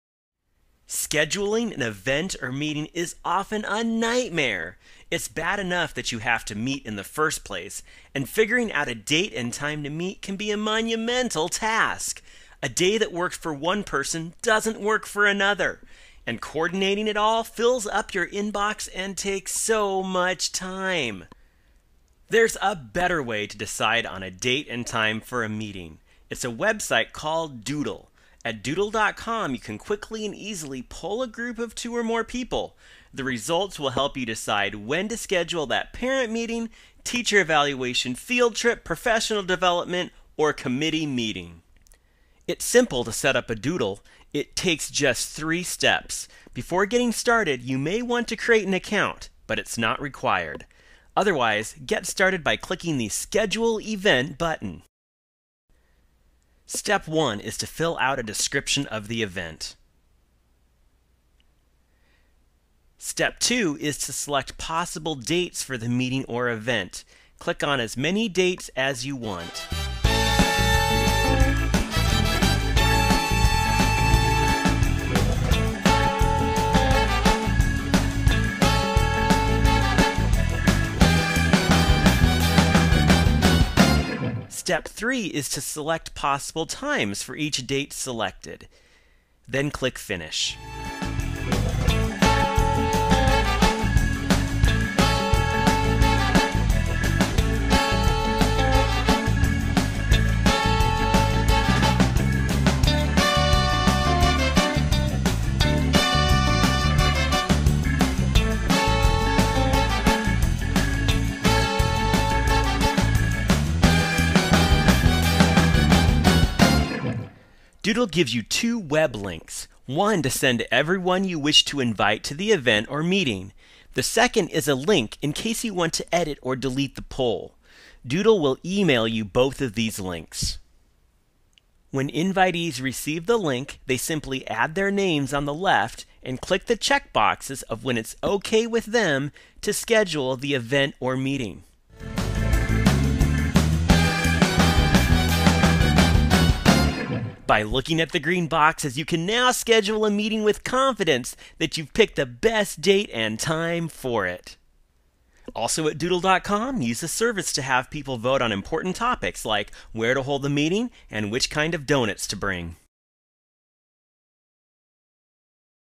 Doodle.com Infomercial
Learn how Doodle.com can help schedule a meeting or event.